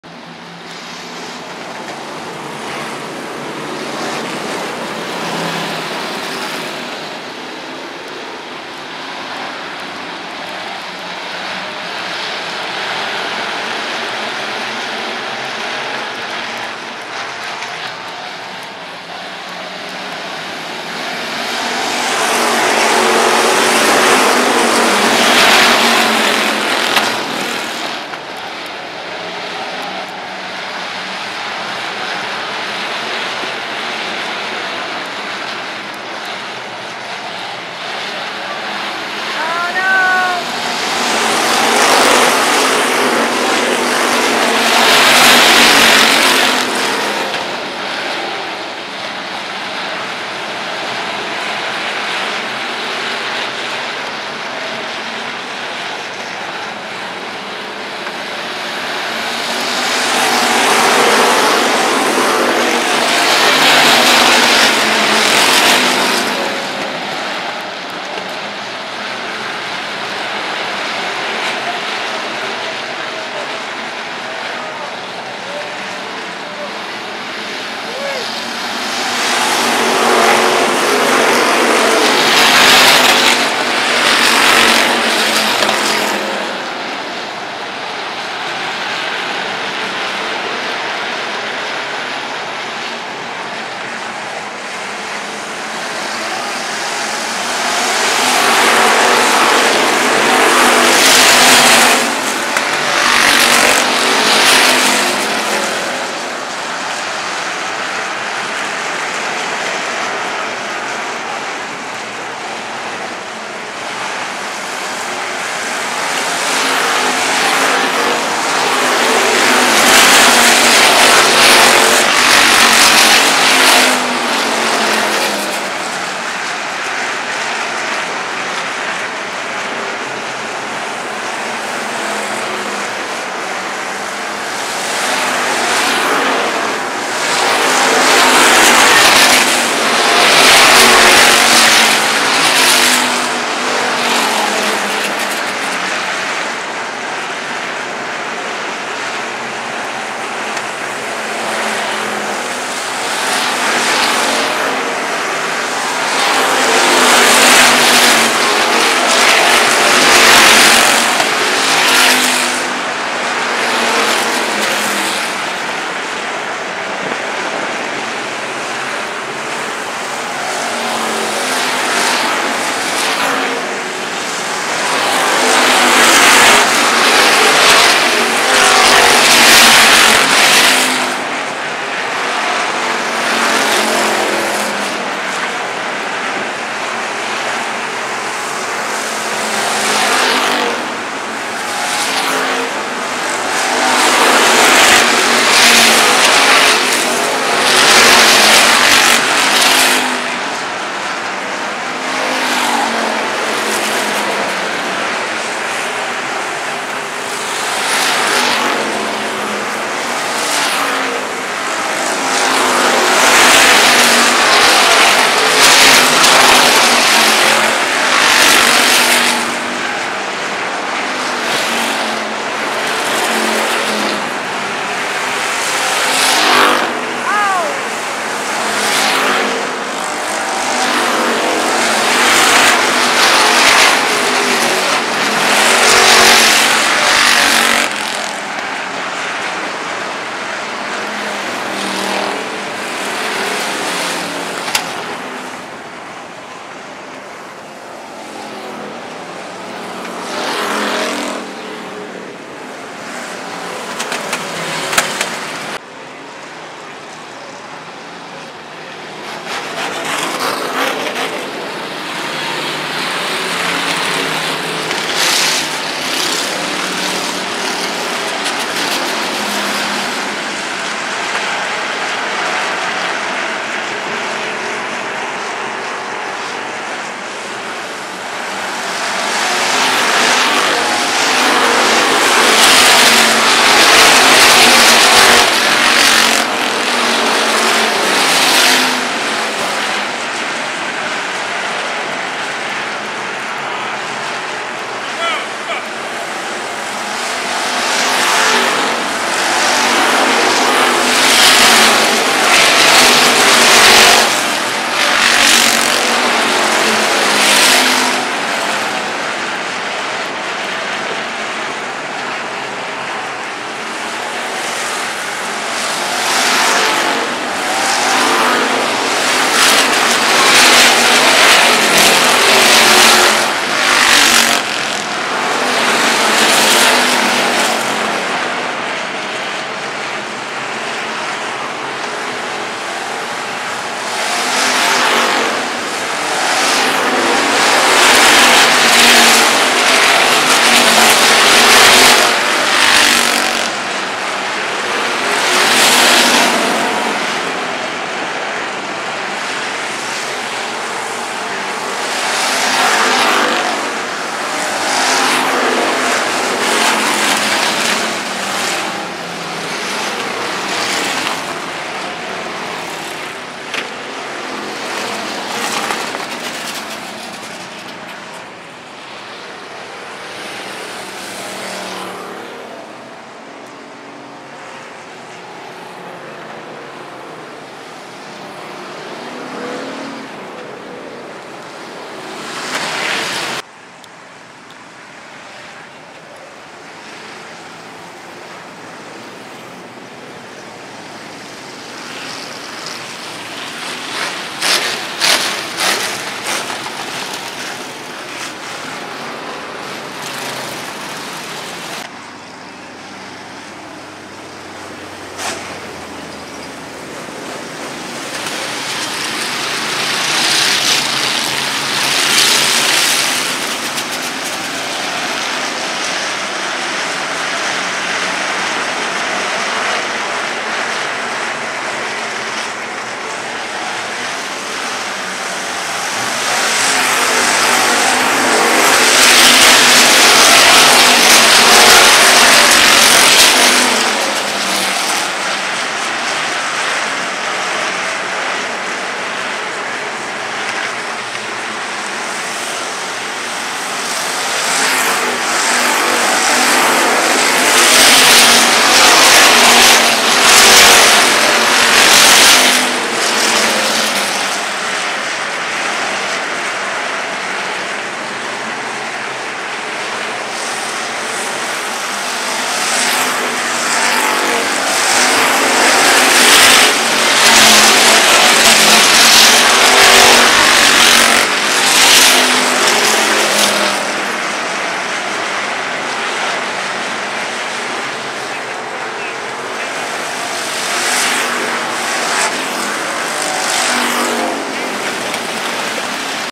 Hobby Stock A Feature @ Eagle Raceway 08.03.2024
Neil Pella 3N starts P16 after racing to a win in the B Feature. Watch as Neil works his way through
the field to cross the checkers in 7th!  Good Run!  Congratulations to Adam Wasserman #41 on the
win!!